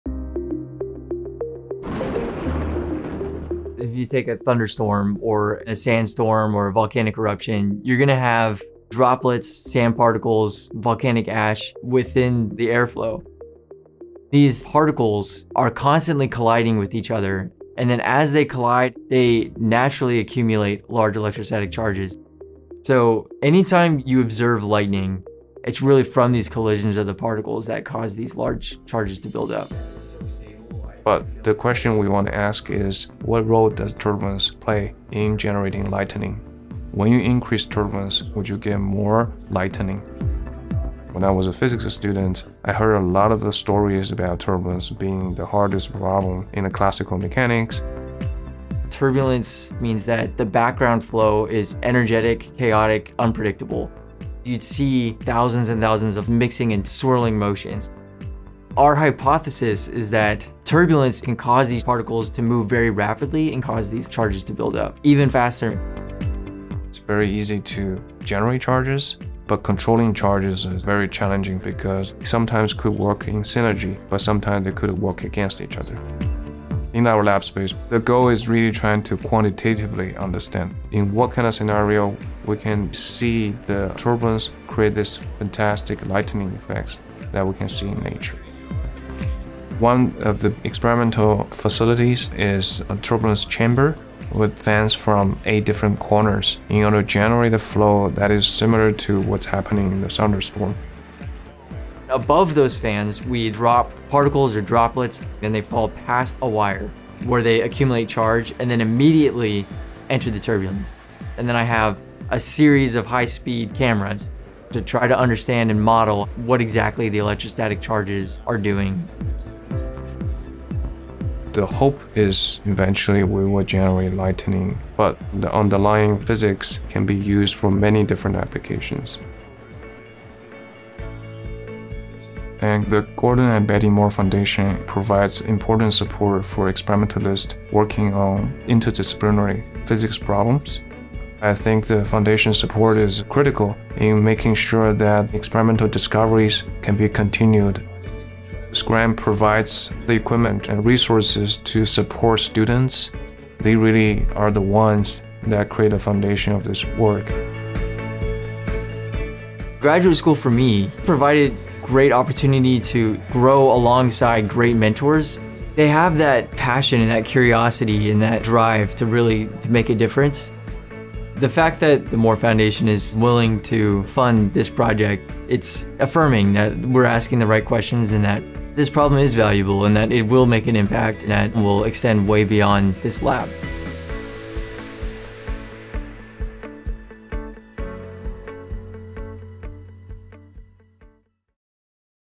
A Spark in Storm Research at Johns Hopkins
What do thunderstorms, volcanic eruptions, and dust storms have in common? They all produce turbulent conditions that can cause lightning, as collisions between tiny particles like dust, ash, or ice lead to a buildup and release of electrical energy.

Supported by the Gordon and Betty Moore Foundation, Whiting professor Rui Ni and his research team are reproducing electrical storms in their lab to study how turbulence, or chaotic air movement, could increase the frequency of lightning. Ni says this work could help better predict lighting strikes, protect aircraft, prevent wildfires, and understand climate patterns.